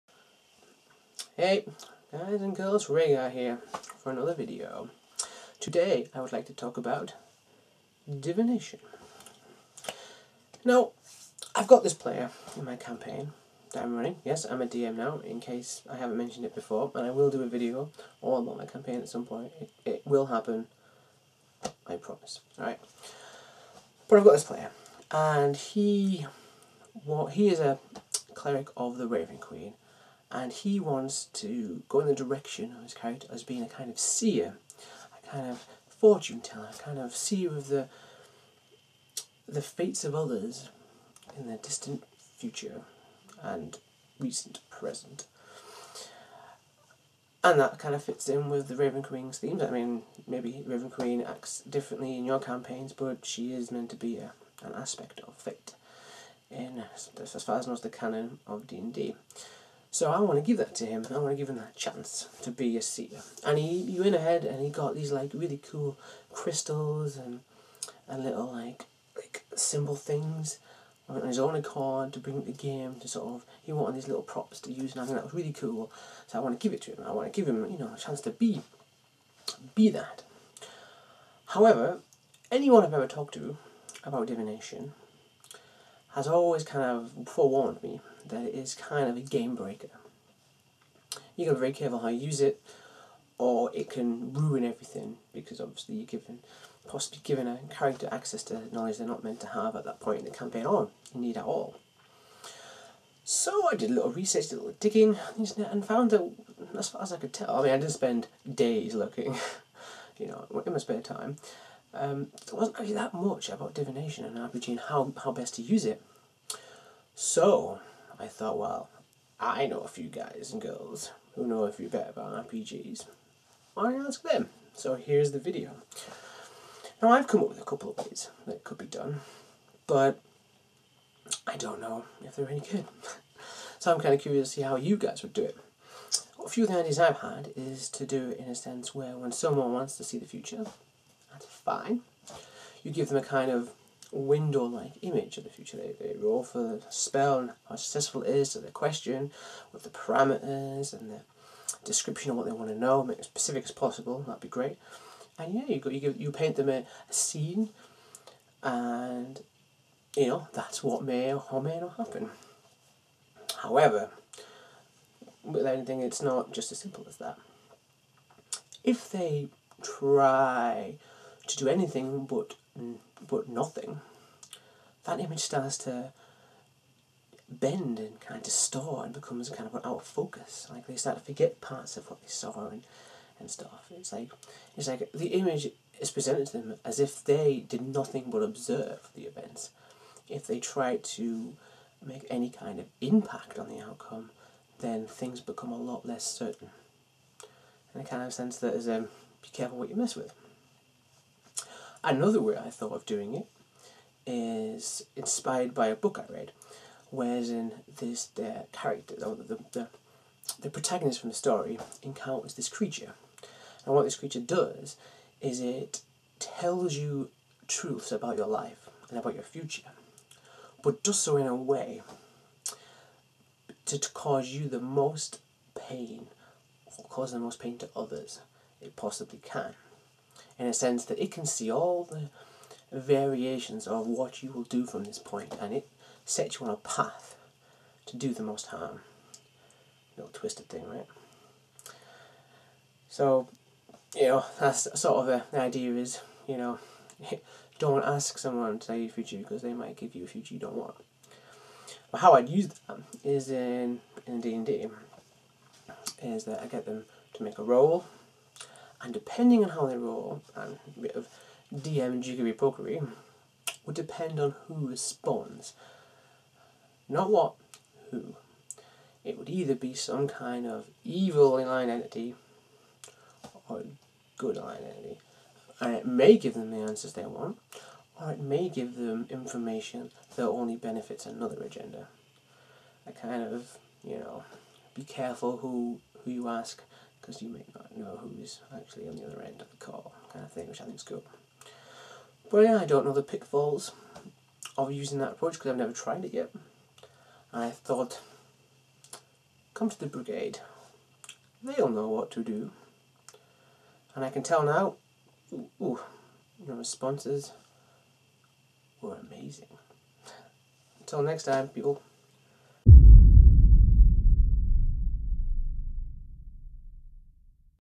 I See what you did there: Divination
A Question Rhaegar poses in regard to the noble art of divination in role playing games.